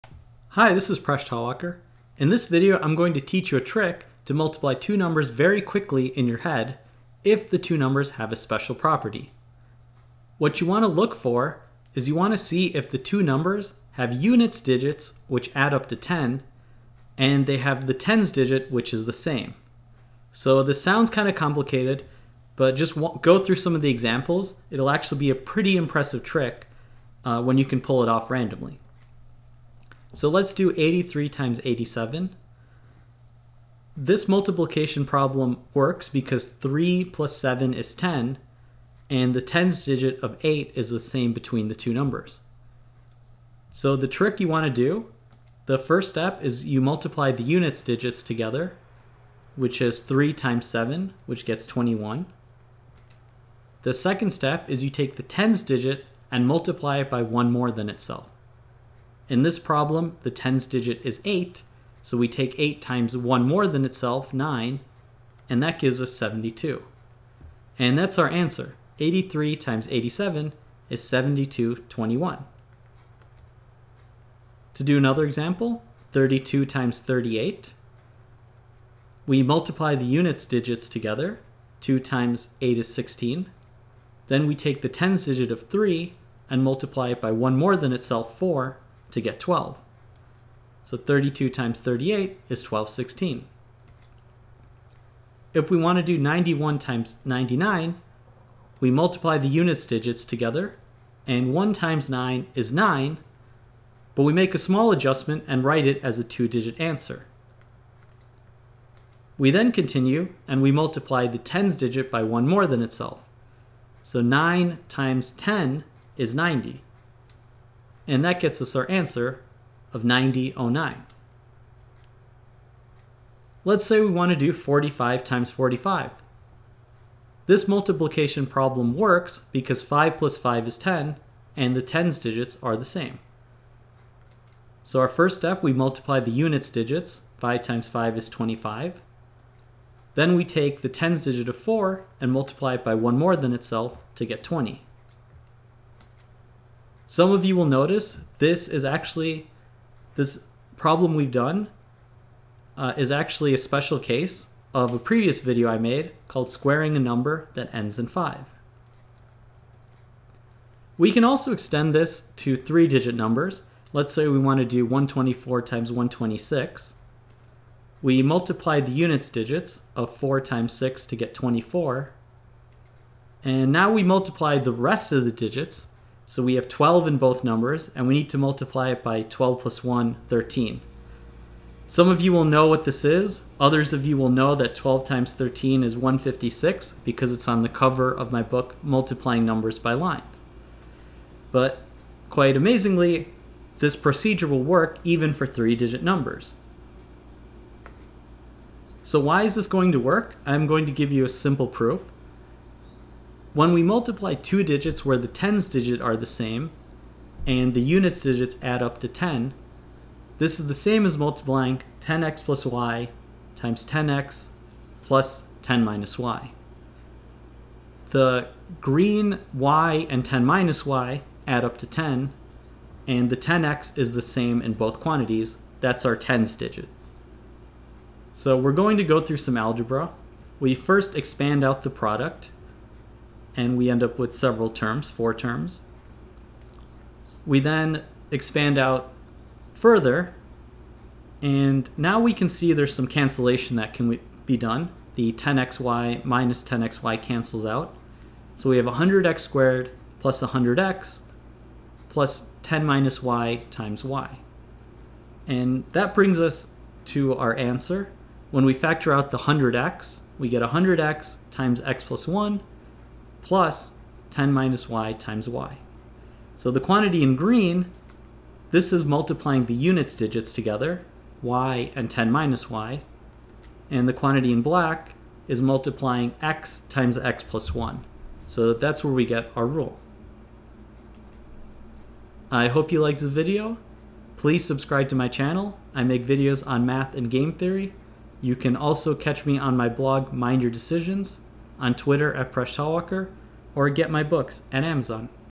How to multiply special pairs of numbers in your head
You'll impress everyone at the party with this mental math trick.

My Books